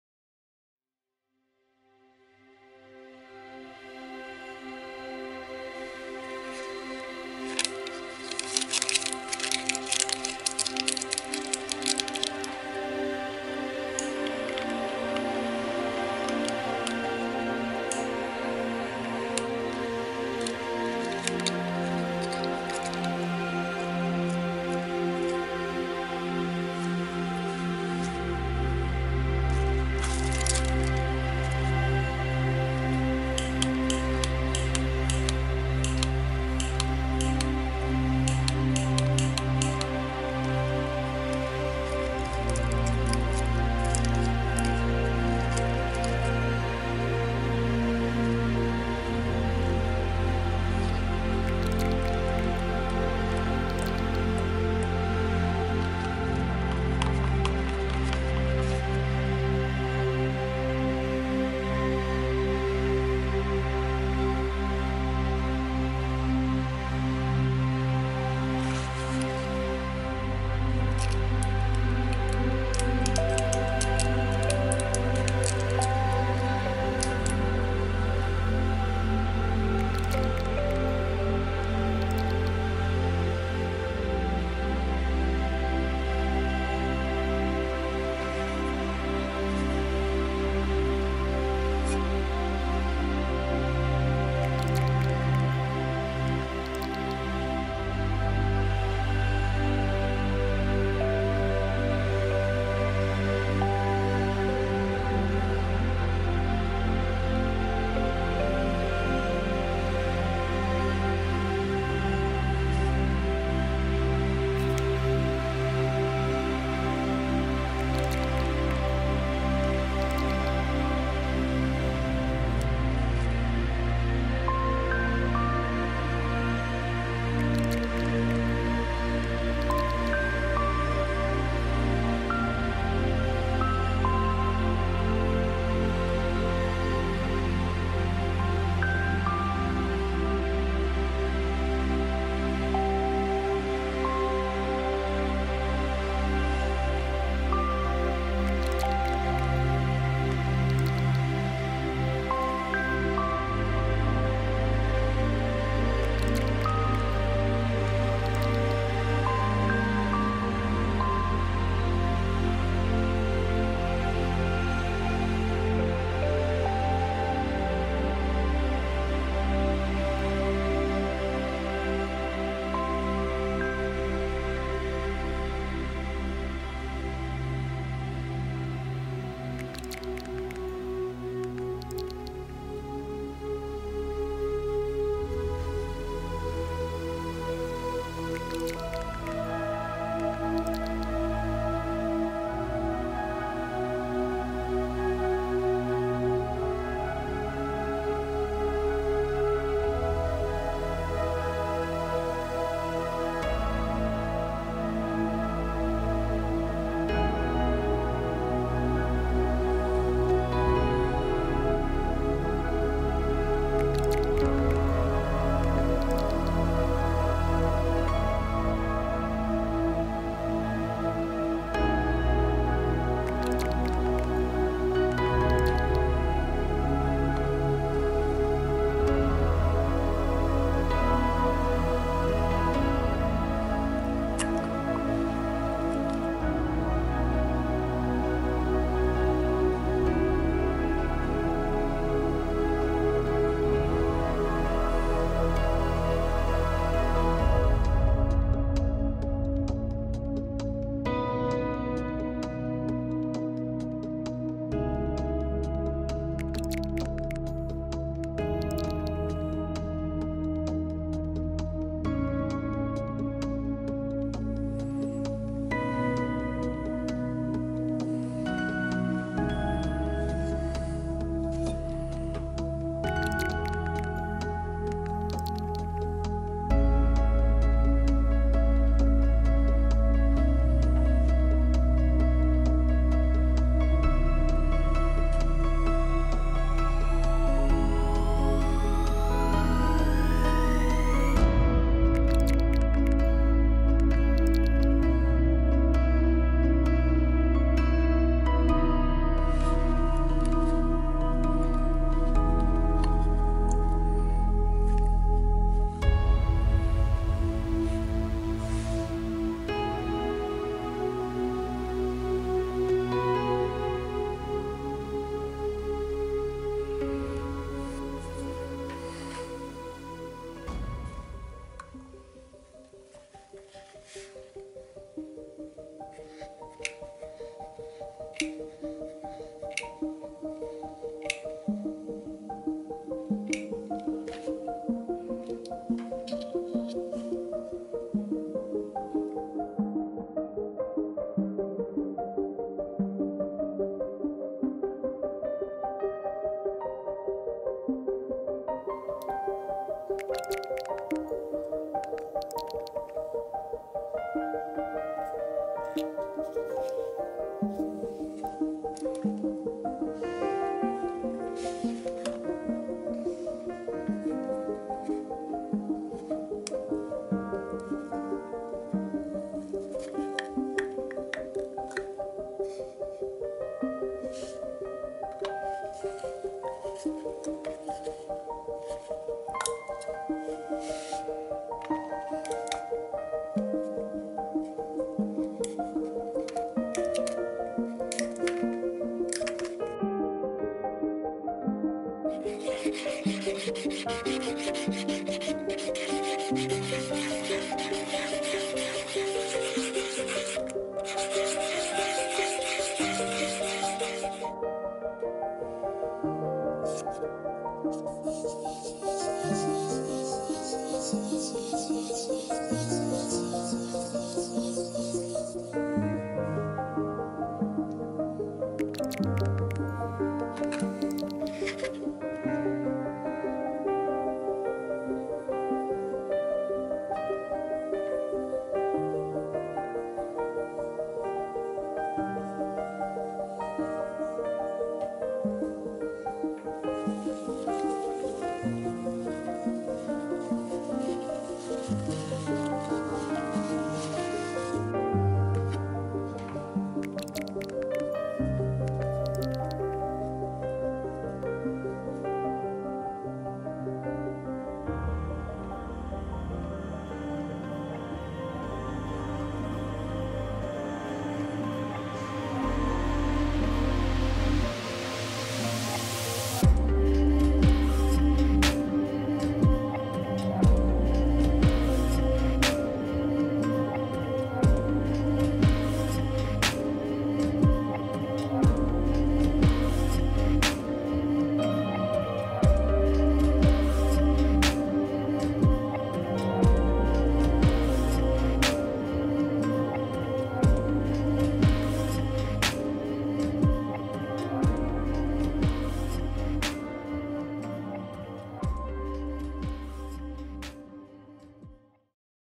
Resin sphere. Rings with acrylic paints in an epoxy resin sphere. Epoxy resin
In this project, I made a resin art sphere. For the lines I used gold, white, mother-of-pearl and silver acrylic paints. I painted the rings in layers and filled with epoxy. After placing the ball in the center, it looks like a planet. The result is space in a sphere.